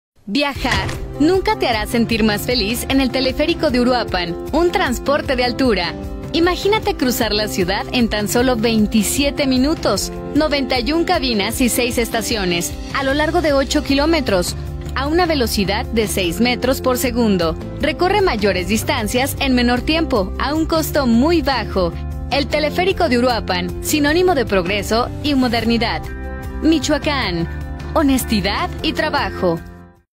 ¿Te imaginas cruzar Uruapan en tan solo 27 minutos mientras disfrutas de una impresionante vista?
¿Te imaginas cruzar Uruapan en tan solo 27 minutos mientras disfrutas de una impresionante vista? ¡Esto será posible gracias con el nuevo teleférico!